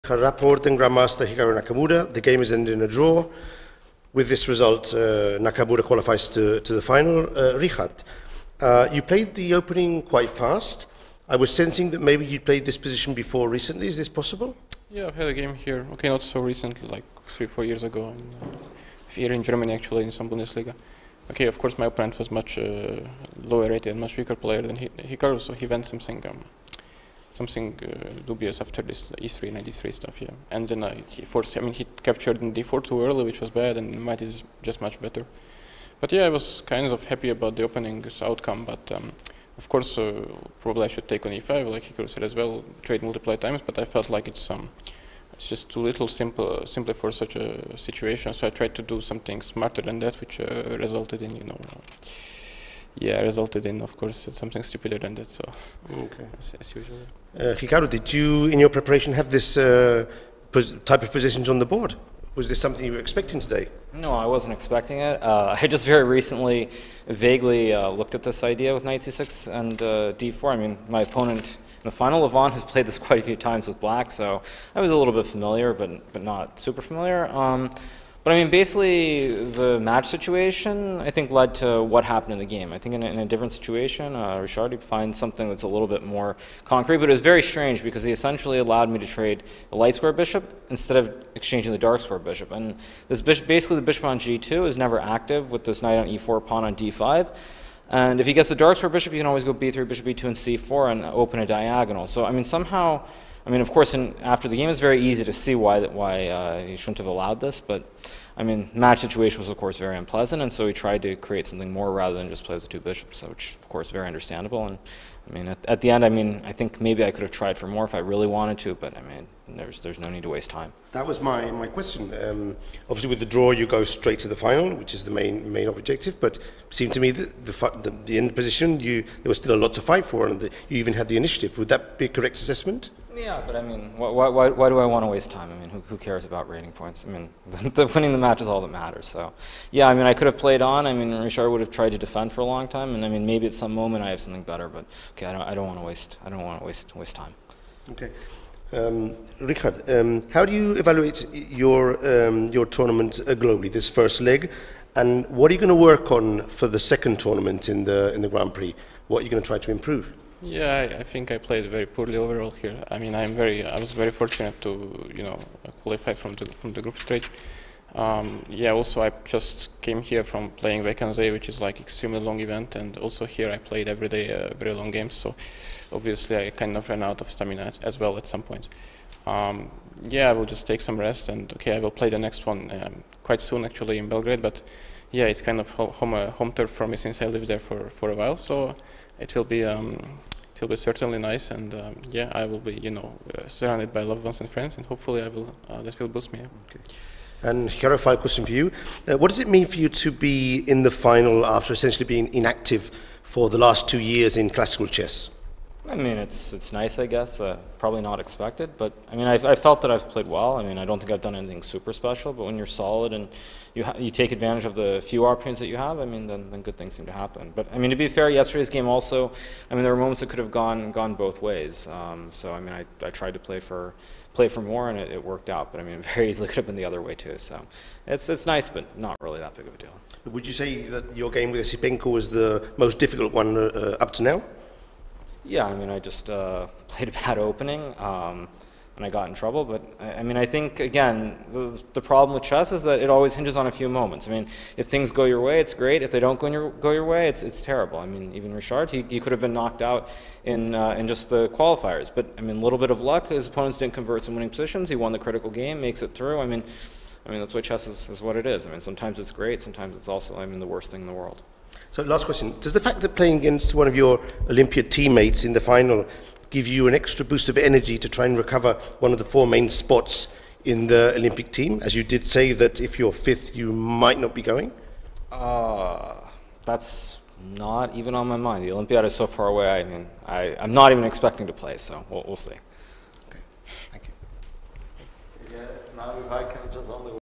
Hikaru Nakamura and Richard Rapport after the semifinals of the FIDE Grand Prix 2022
The FIDE Grand Prix 2022 is a series of three chess tournaments and the final qualifier for the Candidates Tournament. The top two finishers will qualify for the Candidates Tournament 2022, which is the final qualification stage for the World Chess Championship 2023. It’s organized by World Chess. The broadcast is commentated by Chess Grandmaster Evgenij Miroshnichenko and Woman International Master Jesse February.